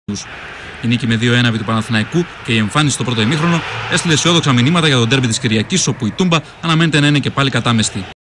Marcin Mięciel PAOK 2-0 Panathinaikos
Marcin strzela brameczke na 2-0